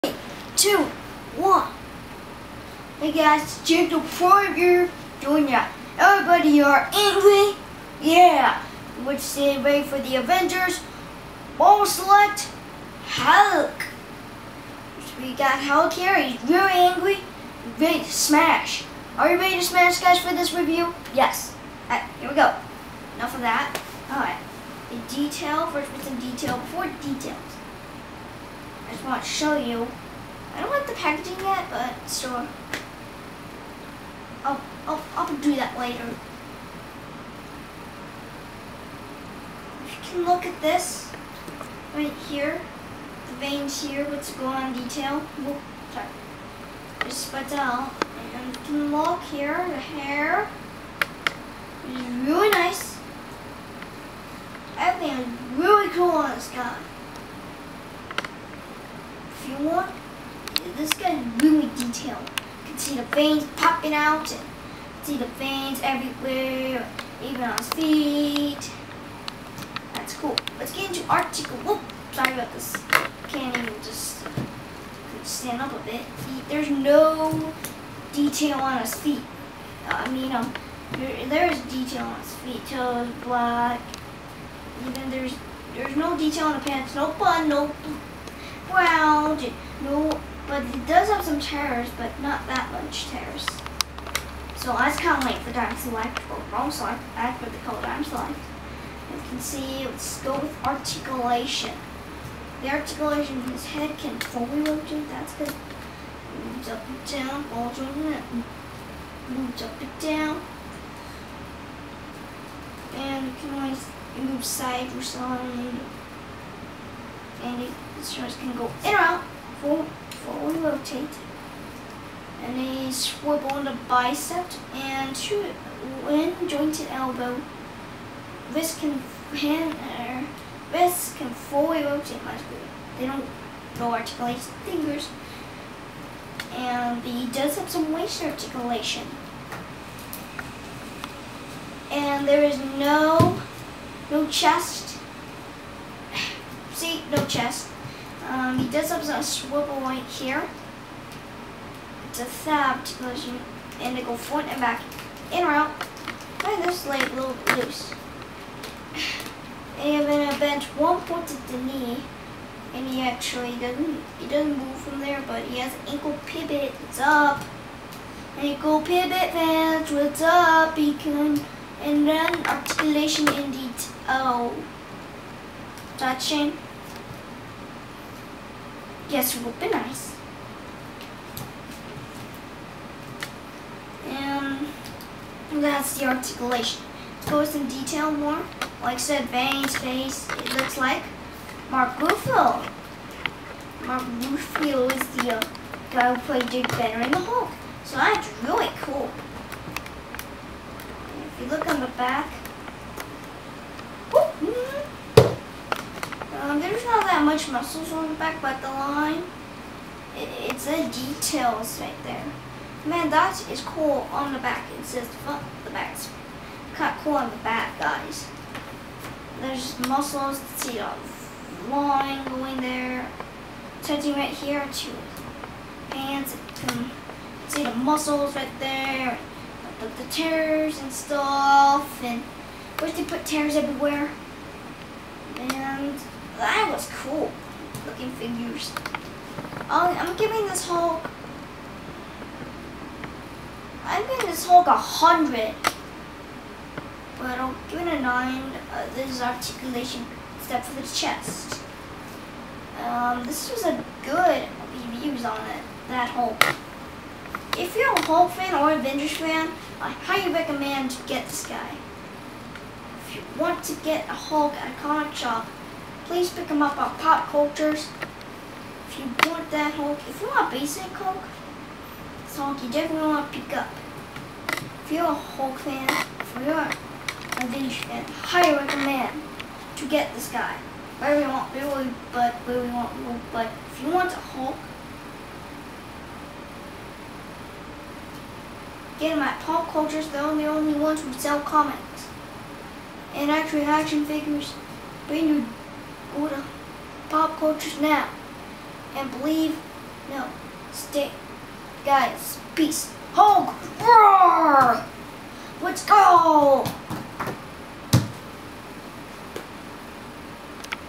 Marvel Select Avengers Movie Hulk Figure Review
Hey guys this is JP with a review of the Avengers Marvel Select Hulk, enjoy!